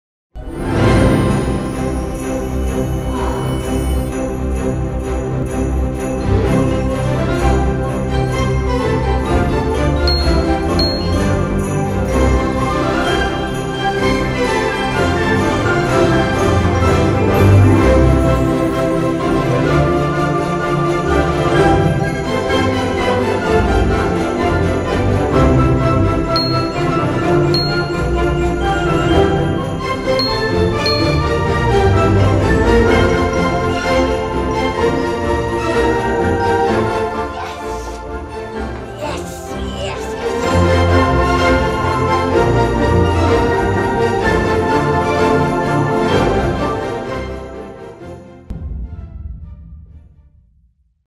Shared Screenings | Vue Entertainment
Our Shared Screenings of Christmas classics give you and your loved ones the chance to experience your favourite festive films on the big screen at the same time, from different venues